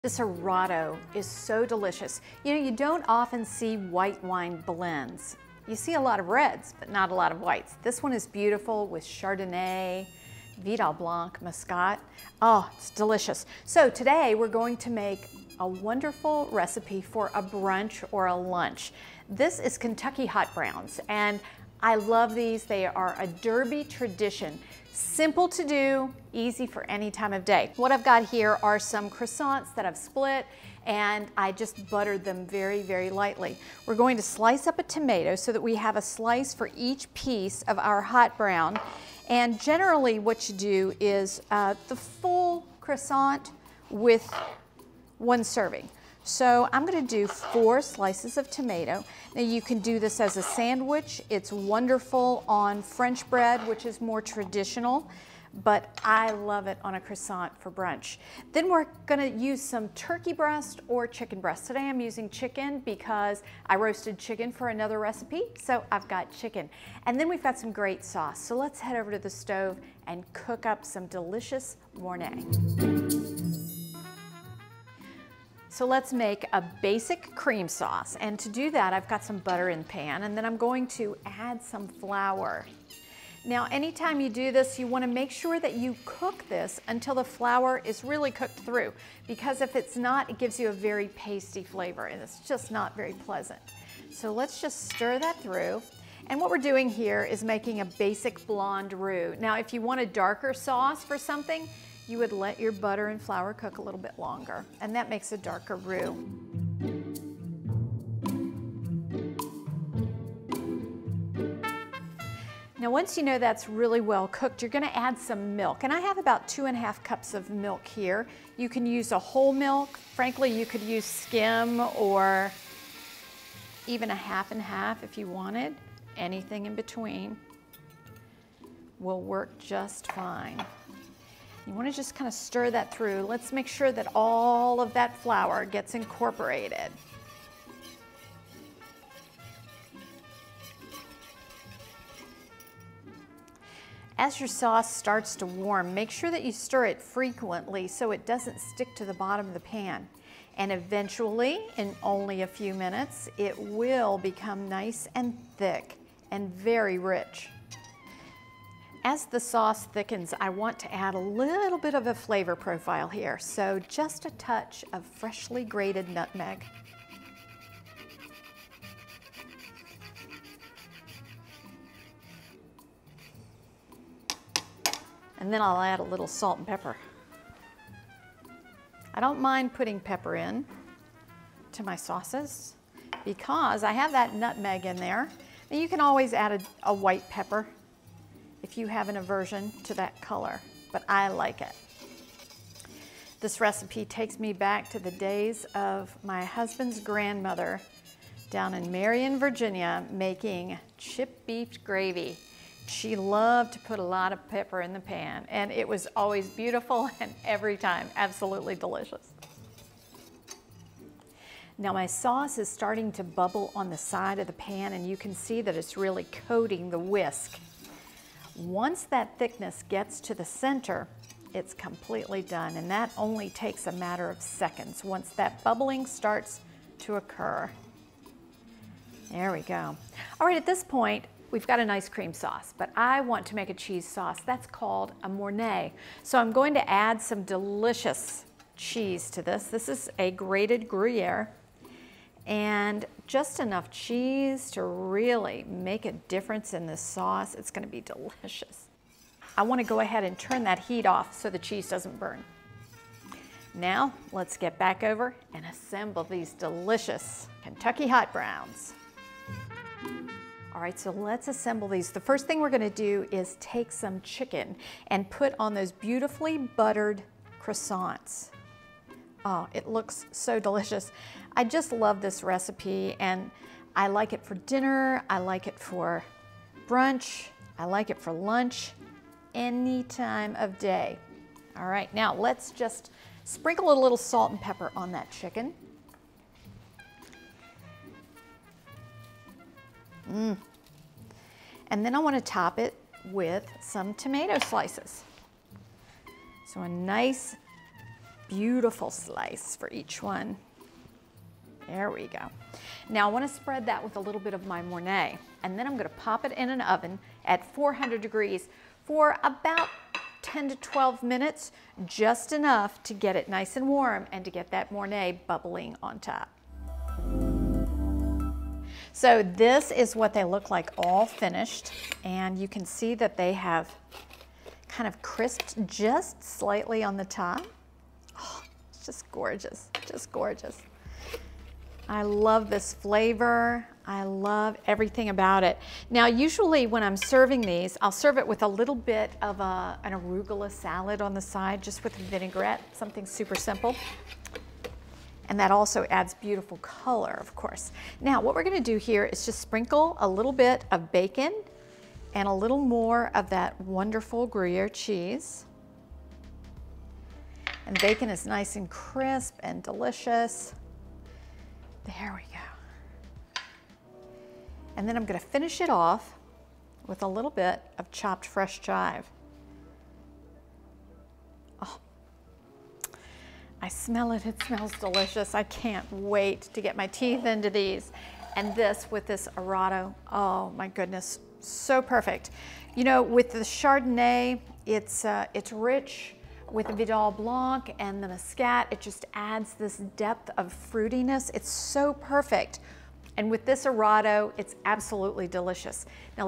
Kentucky Hot Brown Open Face Croissant
A delightful dish for brunch, lunch or a light dinner. This open face sandwich can be made with croissants or French bread and is packed with flavor.  With its creamy sauce atop bacon, tomato and sliced chicken or turkey, this is a classic recipe for the Kentucky Derby.  Change it up with smoked turkey or chicken for even more flavor.